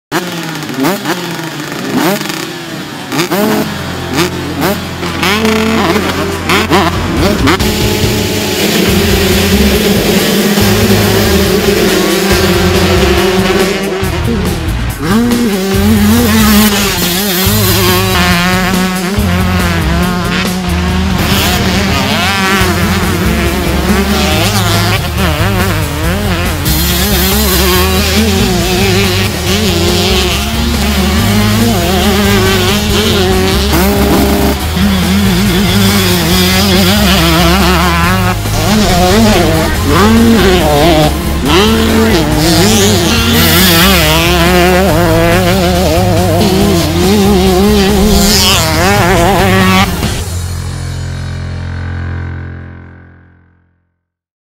The DPH Yamaha Team Australia 2017
The DPH Yamaha Team ripping it up the 2-strokes down under! Can't you smell the the Castor 927?